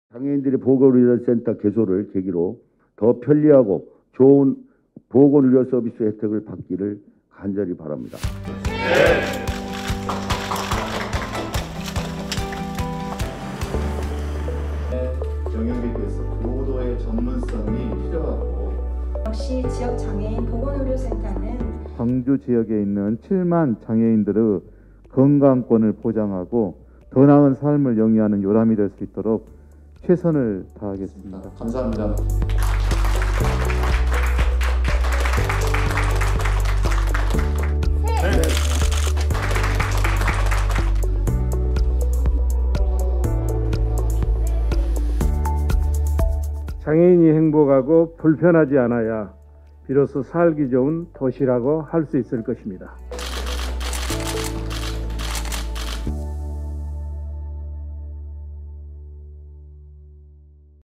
장애인의 건강한 삶이 시작됩니다!
전남대병원에 문을 연 지역 장애인 보건의료센터!
장애인들이 더욱 건강하게 살 수 있는 세상을 바라봅니다~